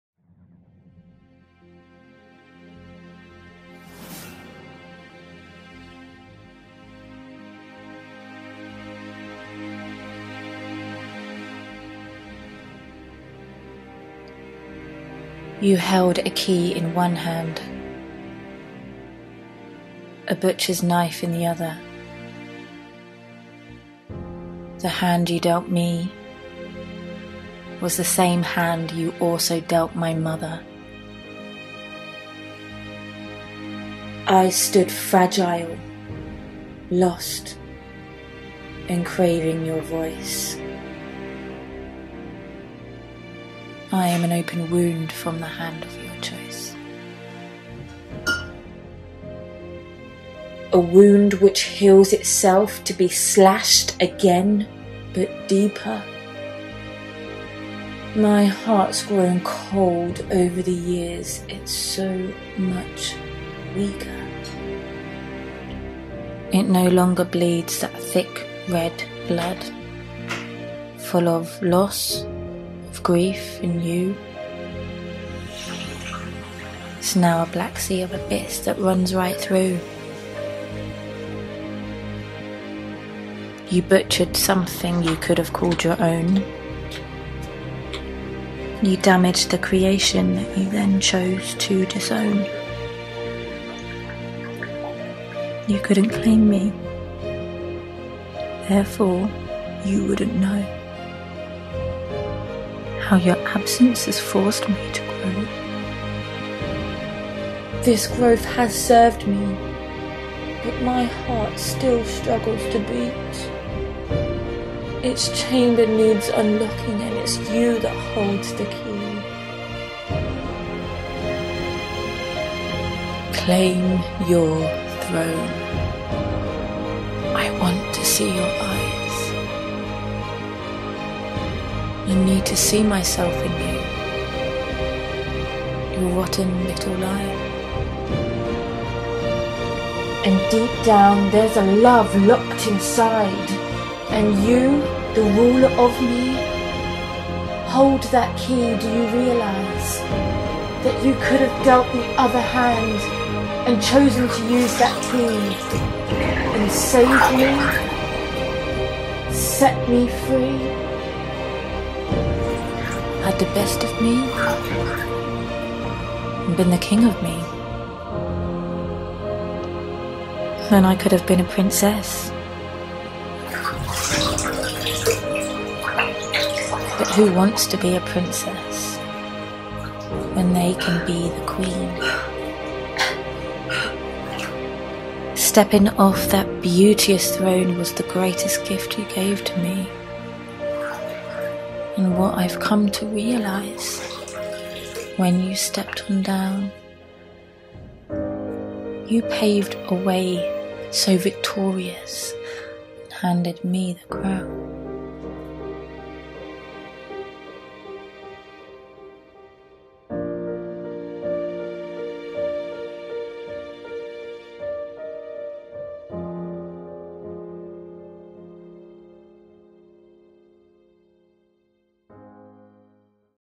Claim Your Throne BY Roxanne Simone
A poetry-film titled Claim Your Throne produced by Urban Gem Media.

A beautiful poem delivered to us by Roxanne Simone, based on an image of claiming an abandoned throne and overcoming darkness, and in doing so illustrates the re-birthing process that takes place within this.

Poet: @RoxanneSimone23
Director/Editor: @AngieBagot
Composer: Michael Maas @Gaijen83